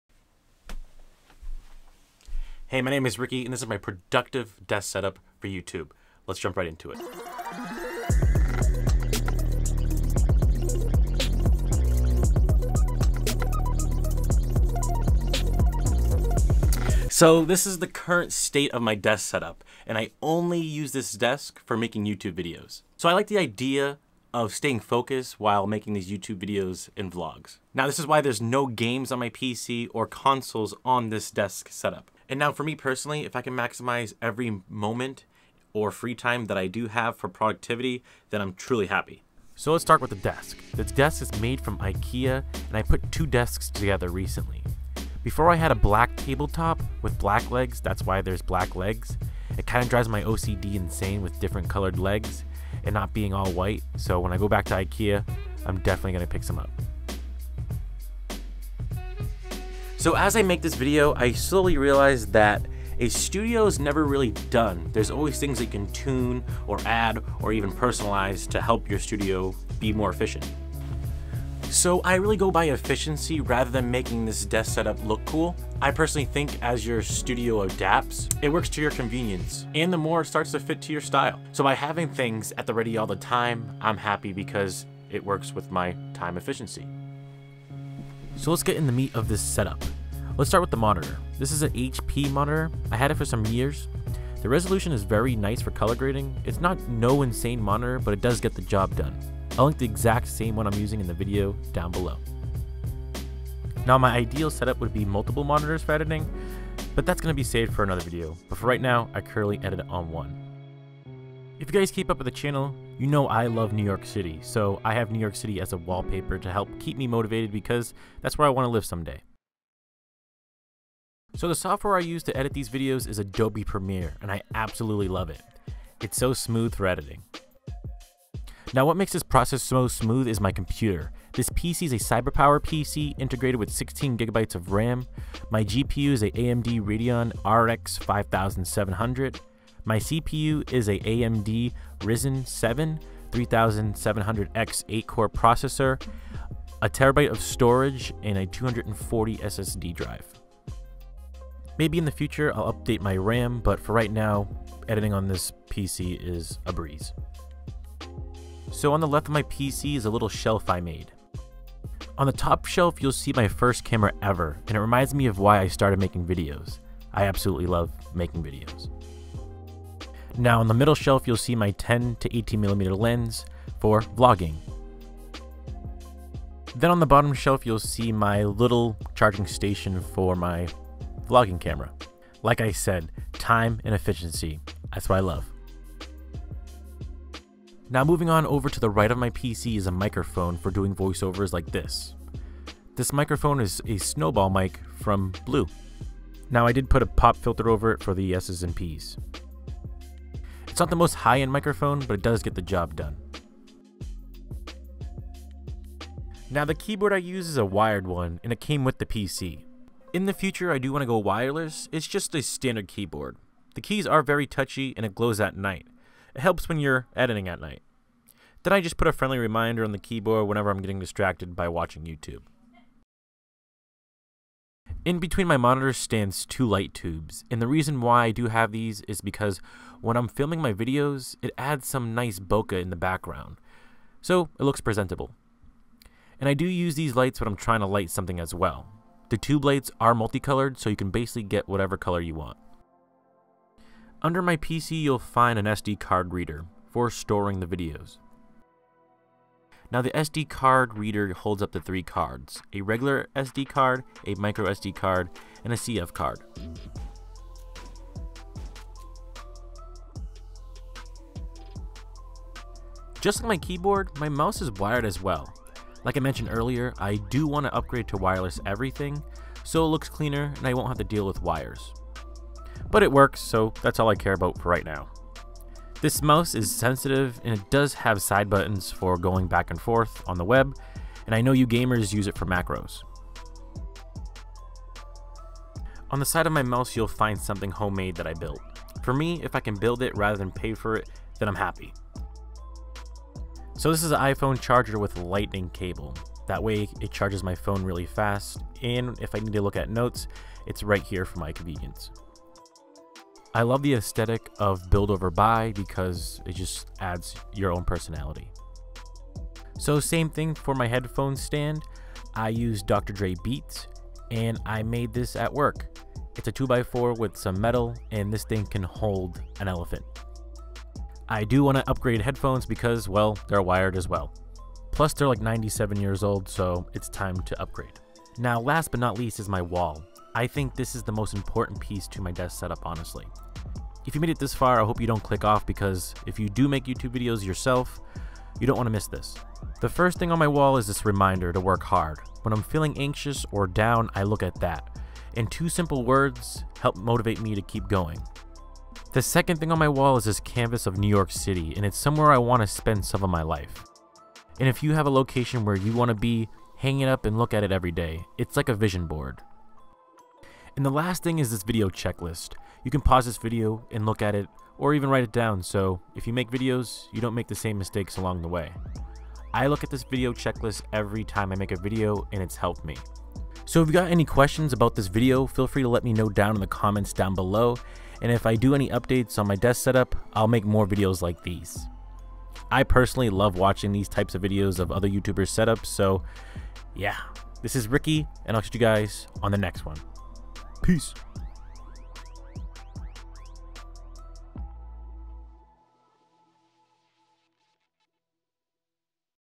My PRODUCTIVE DESK SETUP for YOUTUBE!
In this video I'm breaking down my desk setup for 2022! I love being productive so I figured I'd make a video about my desk!
Monitor
HP - 23es 23" IPS LED FHD Monitor

Computer
CyberPowerPC Ryzen 7 3700X 3.6GHz
16GB 256GB SSD + 2TB HDD

Headphones
Dre Beats

Light tubes
Bower 360 Degree RGB & White 19" Wand Light